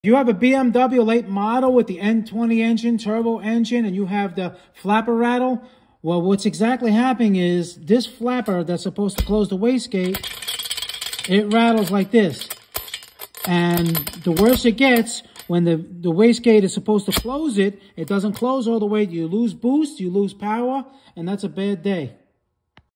BMW Turbo Wastegate Flapper Rattle
No need to replace your entire expensive turbo.
I’ll show you step-by-step how to permanently fix waste gate flapper rattle on most BMW turbos.